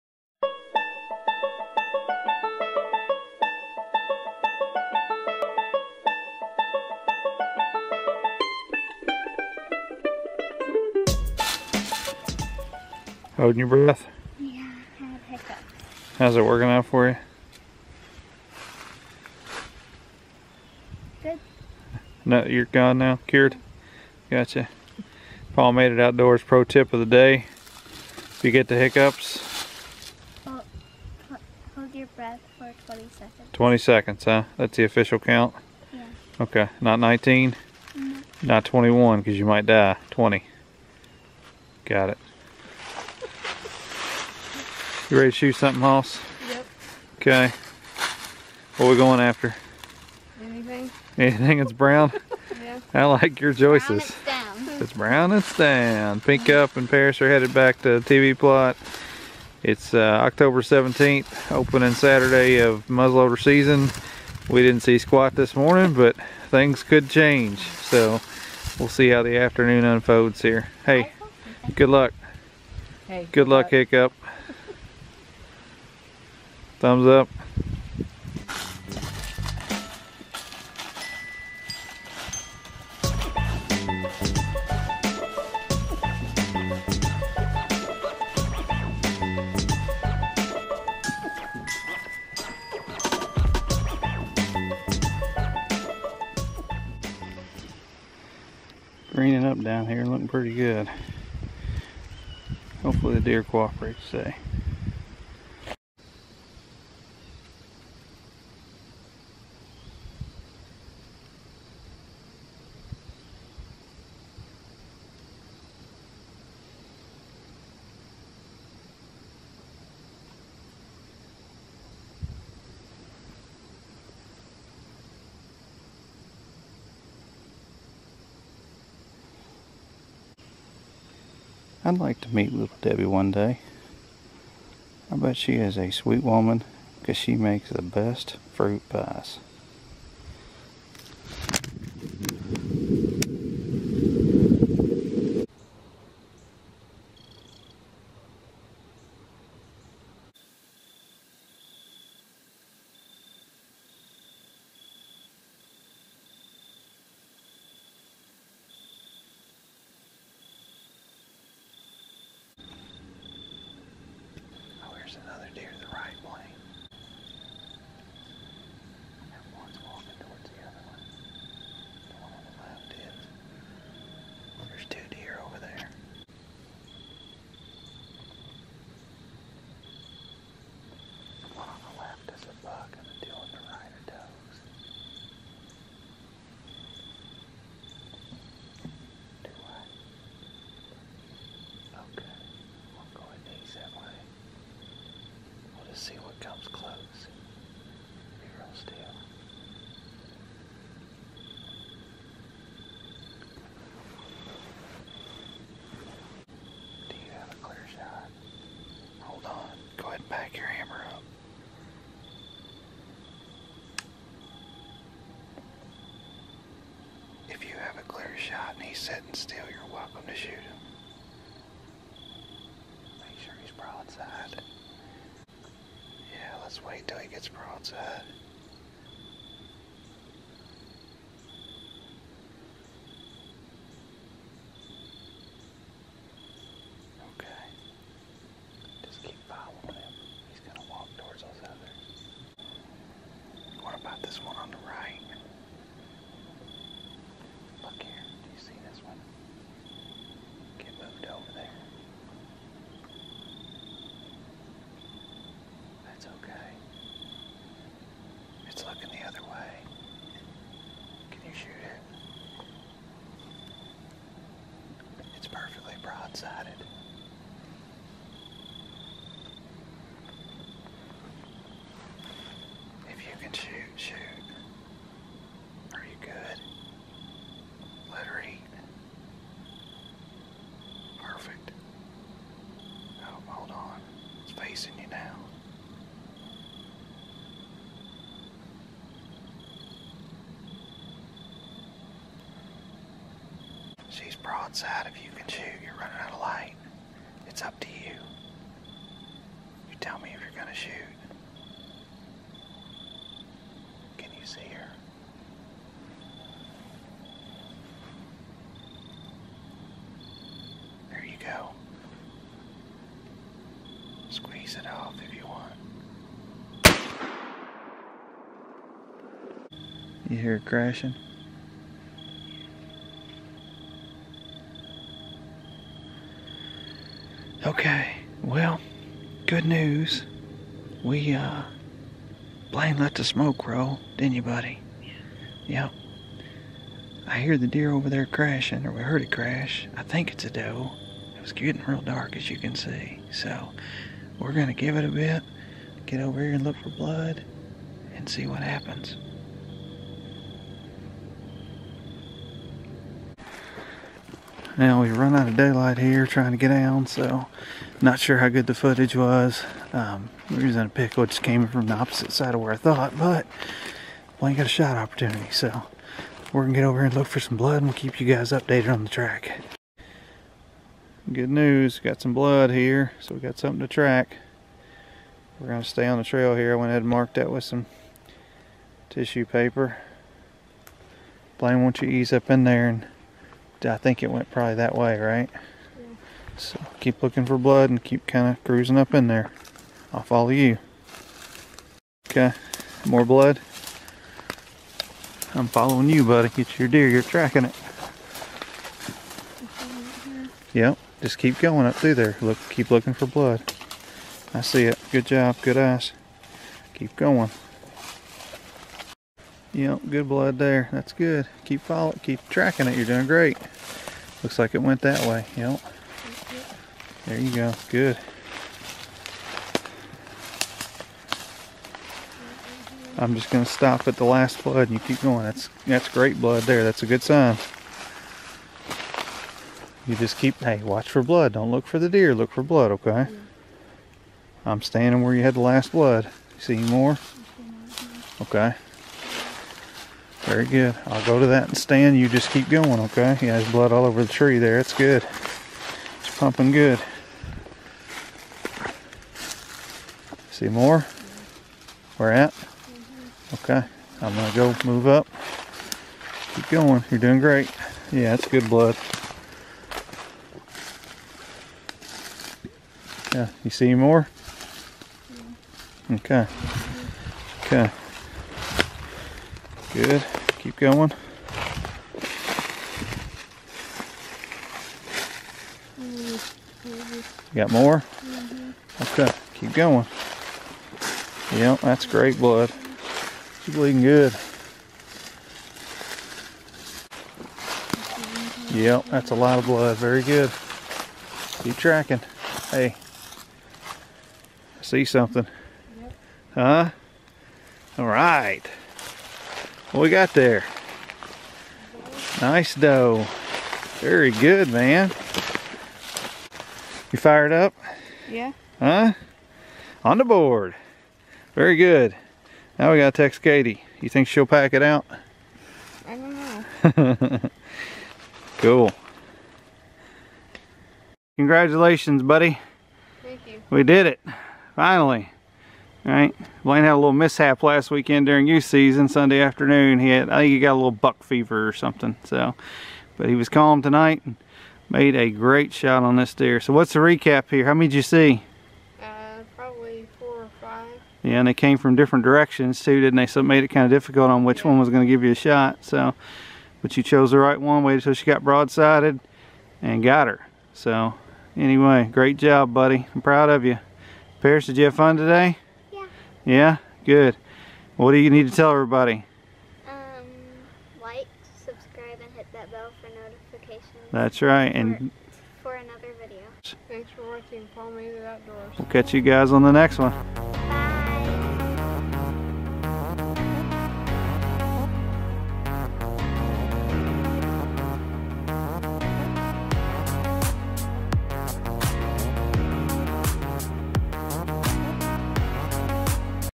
Palmated Outdoors Deer Season '20 Episode 5 Smoke's a Rollin'!
Tags continue to get punched as Blayne harvests a nice doe on this edition of the '20 deer hunting season - smokepole muzzleloader style!